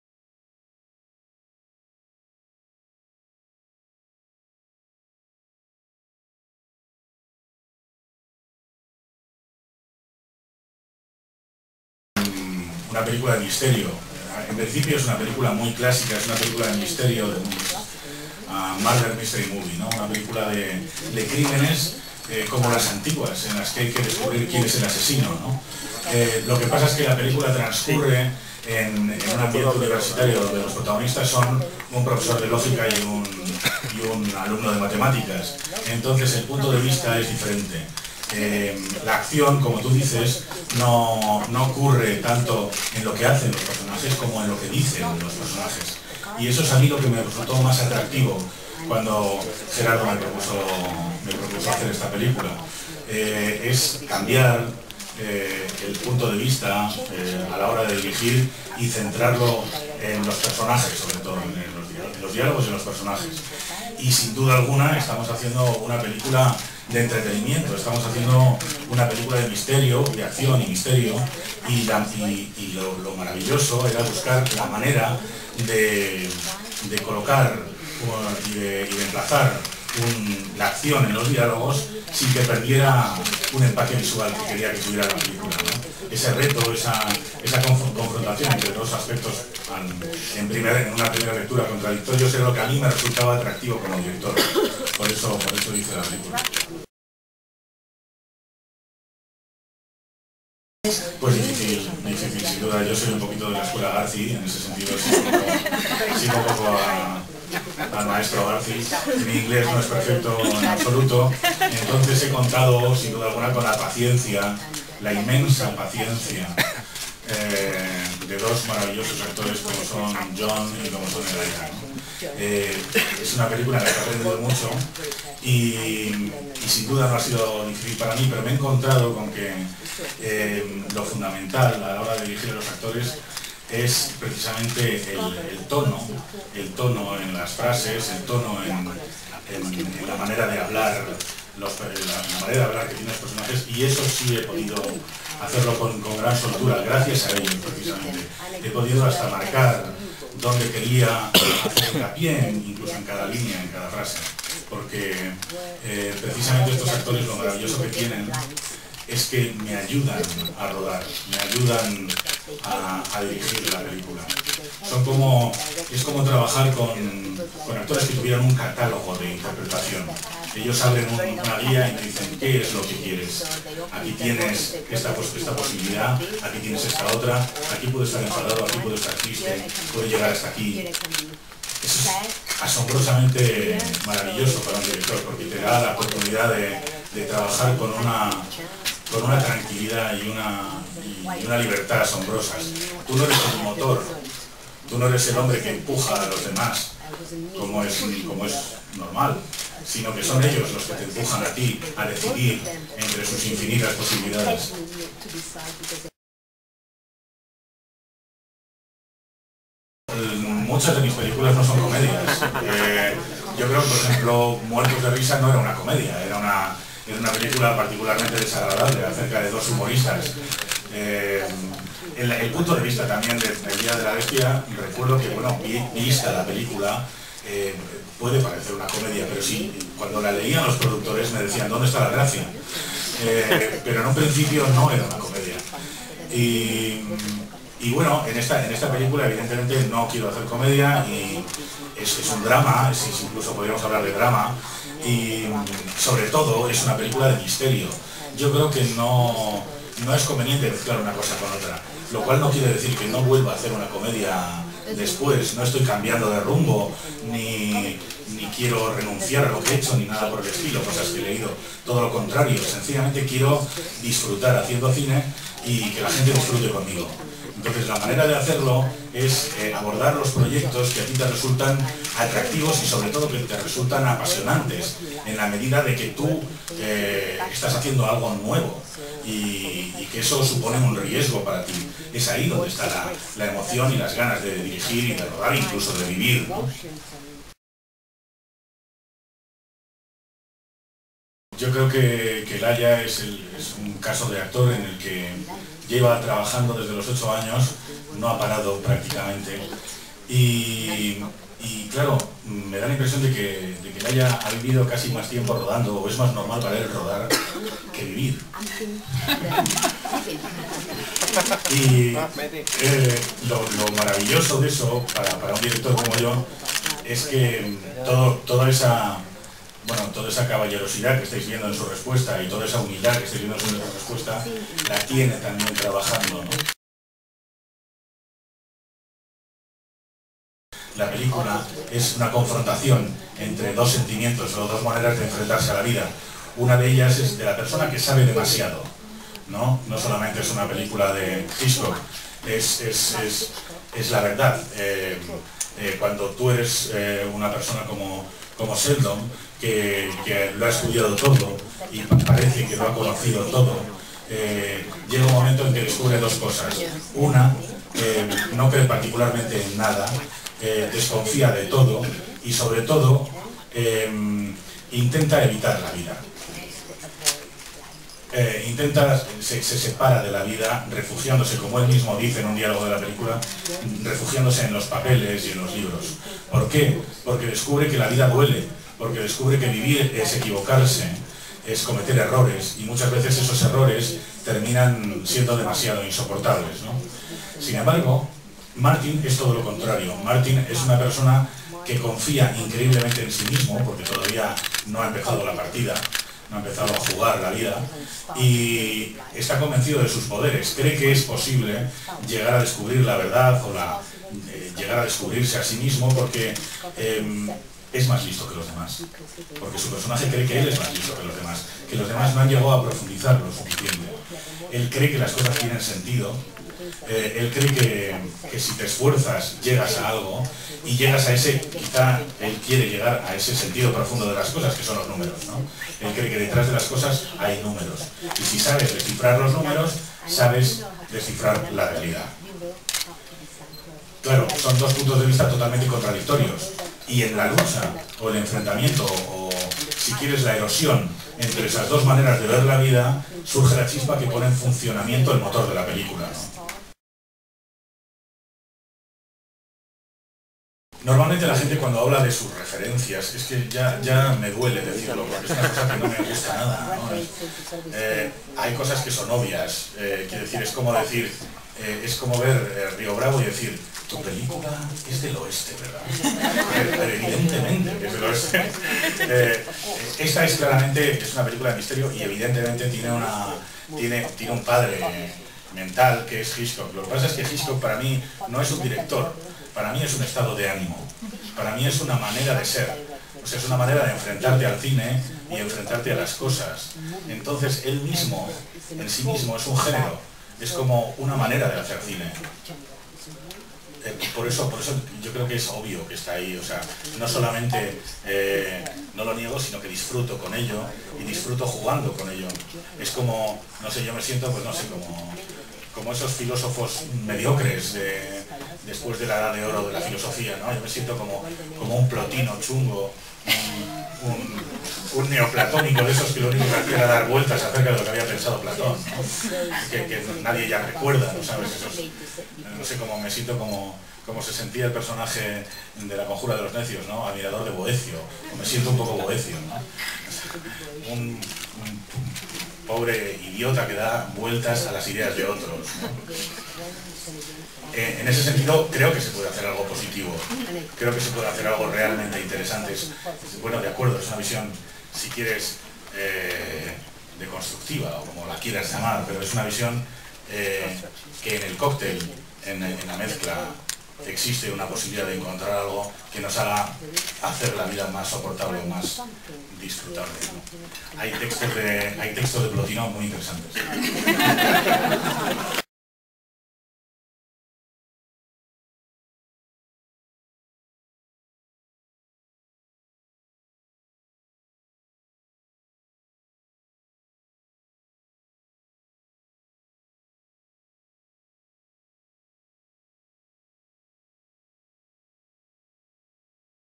LOS CRÍMENES DE OXFORD-PRIMERA PARTE-RUEDA DE PRENSA
Primera parte de la rueda de prensa celebrada en el Hotel Intercontinental de Madrid el 14 de febrero de 2008 con motivo de la presentación del filme a los medios.